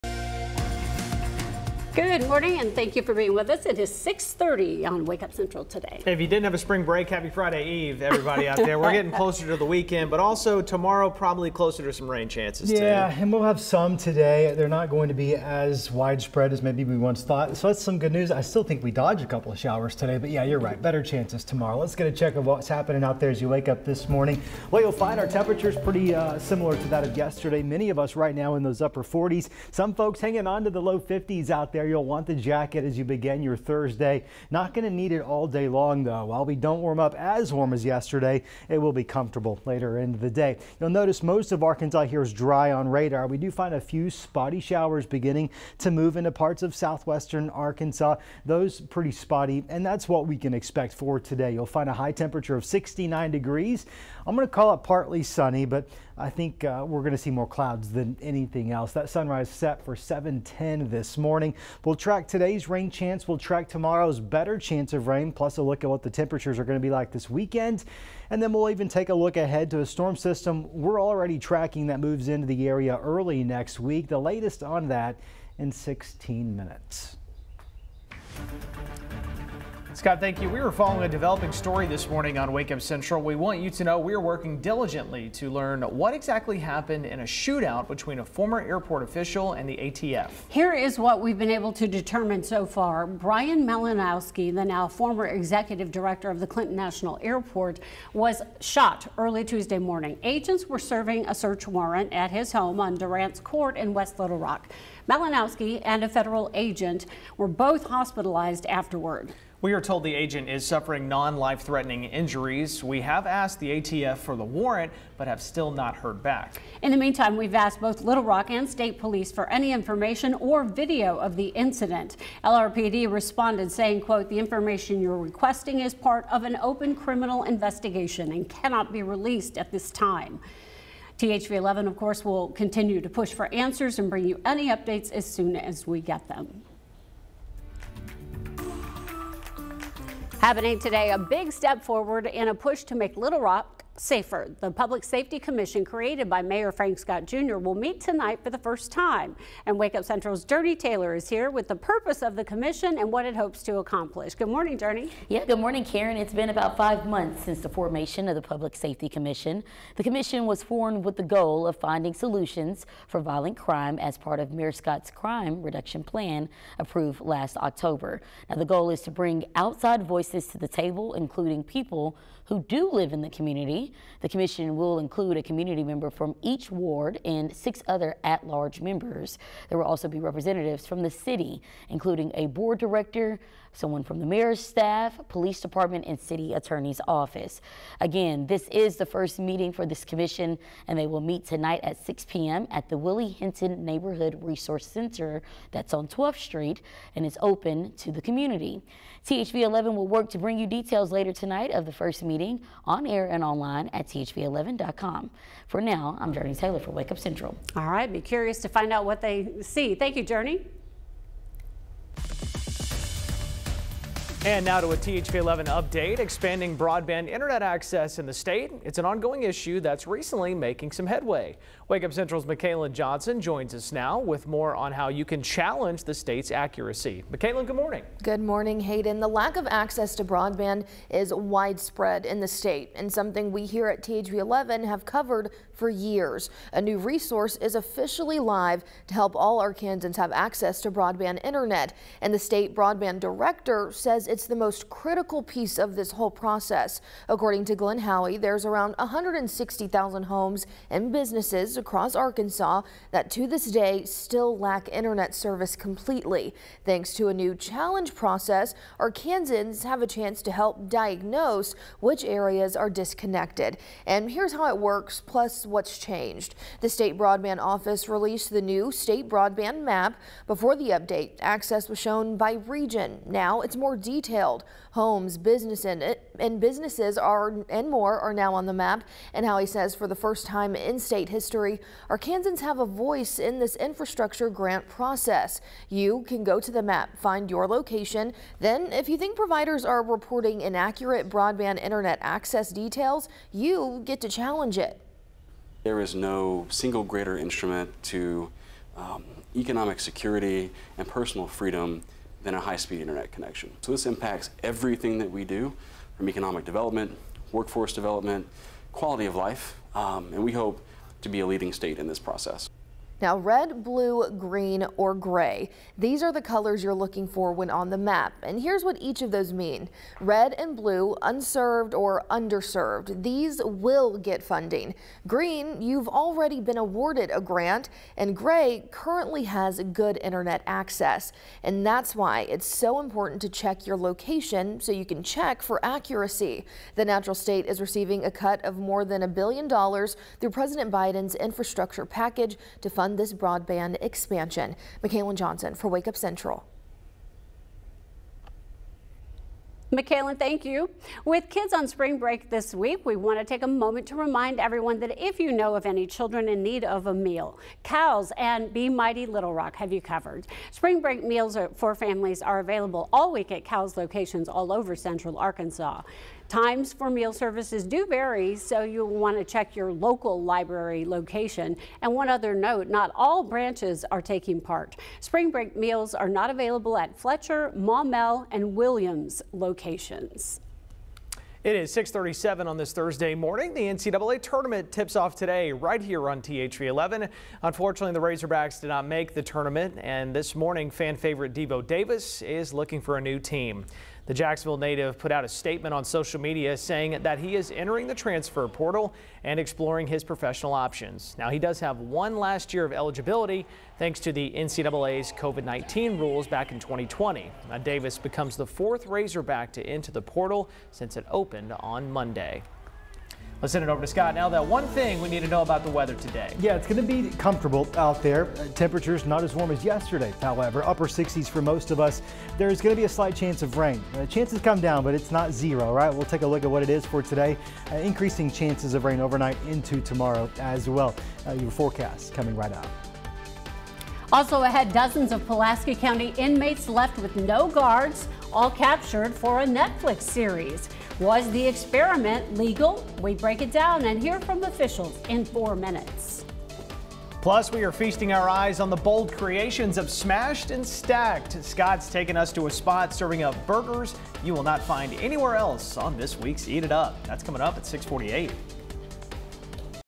Arkansas's top news stories | March 21, 2024
Karen Fuller & Hayden Balgavy present Arkansas's top news stories for March 21, 2024, including updates on an ATF shootout and expanding broadband internet access.